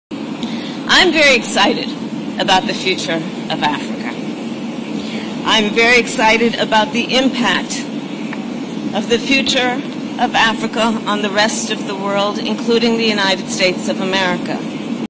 Kamala Harris in Ghana: "I’m very excited about the future of Africa.
Kamala Harris in Ghana: "I’m very excited about the future of Africa. I’m very excited about the impact of the future of Africa on the rest of the world, including the United States of America."